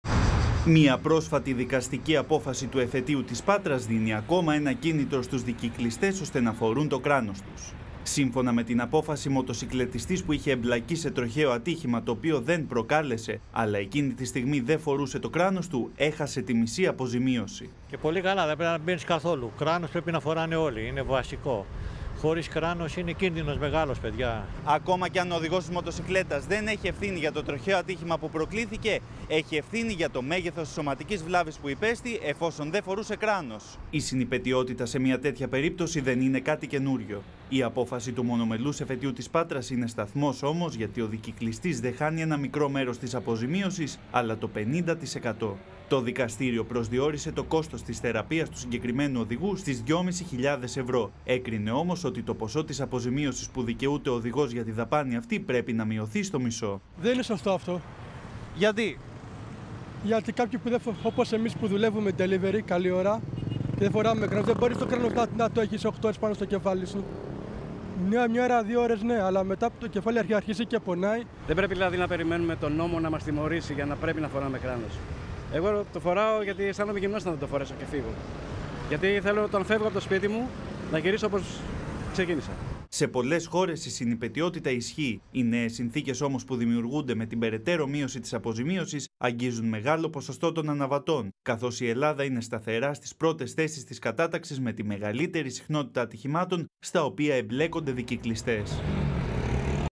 Μειώνεται η αποζημίωση για τροχαία …χωρίς κράνος
Στο μισό θα μειώνεται η αποζημίωση για τους μοτοσικλετιστές που έχουν εμπλακεί σε τροχαία χωρίς να φορούν κράνος, σύμφωνα με πρόσφατη απόφαση του Εφετείου της Πάτρας. Η συνυπαιτιότητα σε μία τέτοια περίπτωση δεν είναι κάτι καινούργιο. Η απόφαση του Μονομελούς Εφετείου της Πάτρας είναι σταθμός, γιατί ο δικυκλιστής δε χάνει ένα μικρό μέρος της αποζημίωσης, αλλά το 50%.